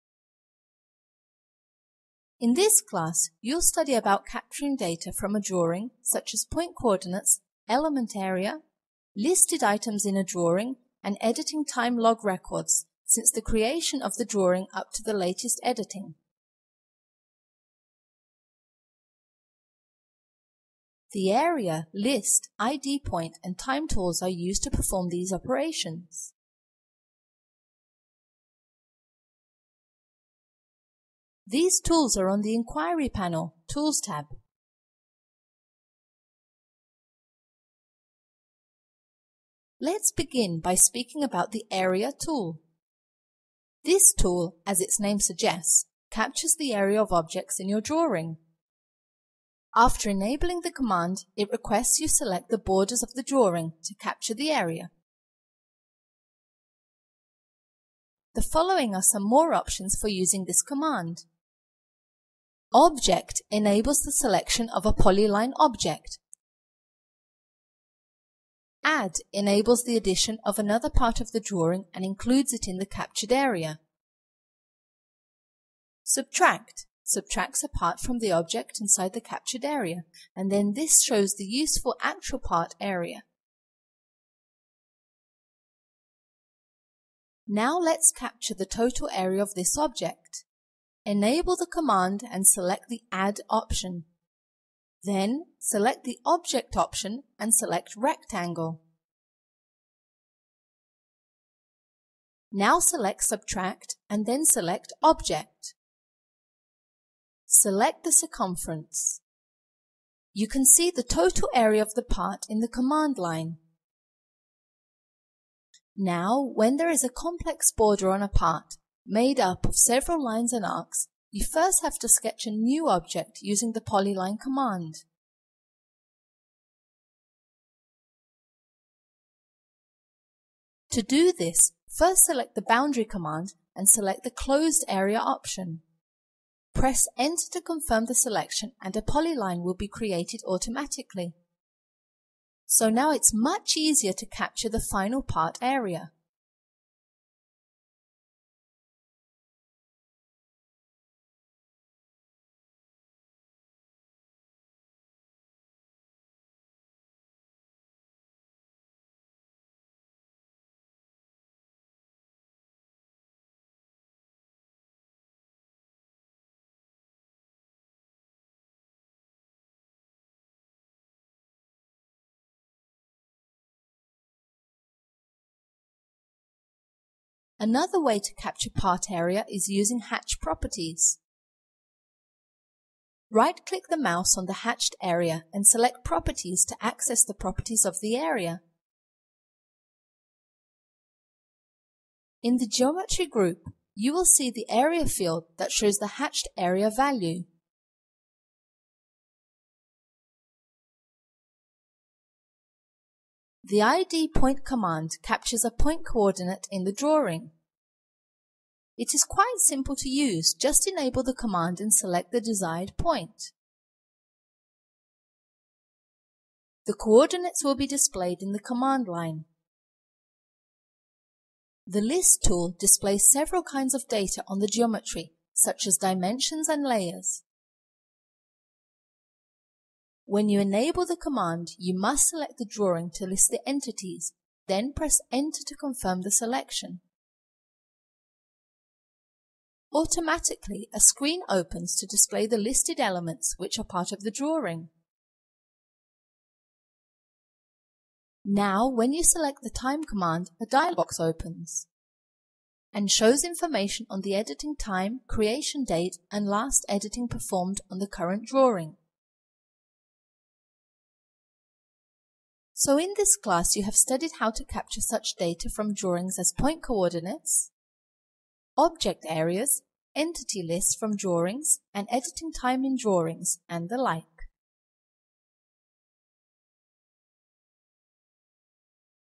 Data Capture Tools - M2A4
In this class, you will study about some tools used for capturing data from drawings, such as element coordinates, areas, and the like.